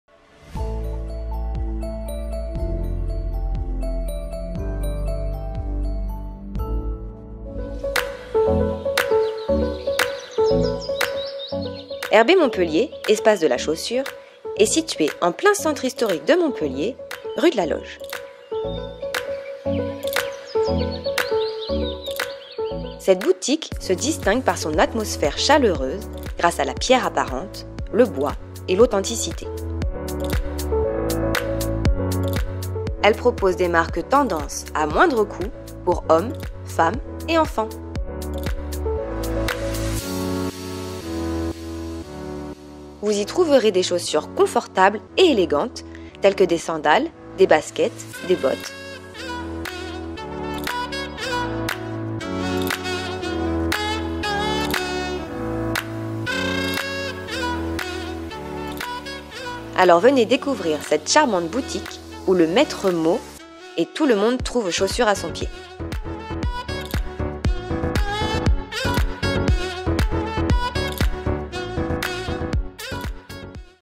Erbe Montpellier Espace de la chaussure | City-Shopping
Erbé Chaussures à Montpellier : la garantie des marques pas chères !

Erbé Espace de la chaussure vous propose des chaussures multi-marques à destination de toute la famille, hommes, femmes et enfants. La boutique se situe en plein centre-ville au 8 rue de la Loge.

Les marques vendues, parmi plus d’une centaine, sont tendance et au meilleur prix. On y trouve les marques suivantes : Remonte, Marco Tozzi, Tommy Hilfiger, Bugatti, Mustang, No Name, Skechers, Timberland, Rieker, TBS… Entre autres !

C’est depuis près d’un demi-siècle que l’enseigne montpelliéraine Erbé vous propose des chaussures de qualité qui font toute la différence : elles sont choisies avec soin parmi les plus grande collections ds créateurs. L’aventure a débuté à Sète, Balaruc et au Cap d’Agde pour les clients les plus désireux de se munir de chaussures très tendance, notamment avec des sandales.

Le gérant Mathieu Bonnafous explique : « Notre ADN, c’est notre indépendance, nous ne sommes pas franchisés donc depuis mon grand-père, dès 1963, nous avons toujours sélectionné des modèles originaux mais élégants et sans tomber dans l’extravagance. Et ce, avec le souci constant dans la famille de veiller à ce que le prix de la chaussure reste très abordable ». Des marques de qualité et bon marché, en somme.

La décoration de l’enseigne se distingue notamment par son atmosphère pleine de clarté et d’espace, mais également très chaleureux en provilégiant le bois et la pierre, et donc l’authenticité.
Quant au fleuron de l’enseigne, Erbé à Montpellier, situé en plein Ecusson (centre-ville historique), le choix a été fait de conserver et de restaurer les belles voûtes médiévales du plafond ainsi que les murs en pierres apparentes. Le mobilier en chêne patiné par les ans forment un écrin inimitable pour les produits à la vente.
En termes de conseil, la gérante montpelliéraine saura vous accompagner pour trouver votre coup de coeur en fonction de vos goûts, de votre âge et des saisons.

Vous y trouverez également un large choix dans la gamme sport et baskets, avec des articles très décontractés parmi les marques Fila, Puma, Adidas, Victoria ou encore Vans.

Mais Erbé chaussures n’oublie pas ses racines, proches de la mer ! Votre magasin vous propose également un grand choix de sandales plates ou à talons, parfaites pour l’été : les très branchées Plakton, Eva Frutos, les tongs Birkenstock ou encore Quicksilver. Sans oublier les Tropéziennes que les magasins Erbé ont été parmi les tout premiers à croire en la marque et à la commercialiser avant qu’elle ne devienne un « must have » pour toutes les modeuses estivales.

Mais ce n’est pas tout ! Erbé complète ses collections avec tout un lot d’accessoires, de produits d’entretien de la chaussure et de lacets, comme chez un cordonnier.

Alors si vous cherchez de quoi vous chausser pour être tendance sans être ruiné(e)s, rejoignez l’ambiance Erbé et faites comme ces nombreuses générations de clients fidèles qui ont apprécié leur slogan, et où « tout le monde trouve chaussure à son pied » !

Accès PMR : oui
Animaux de compagnie : oui
Parking : oui
Modes de paiement acceptés : CB et espèces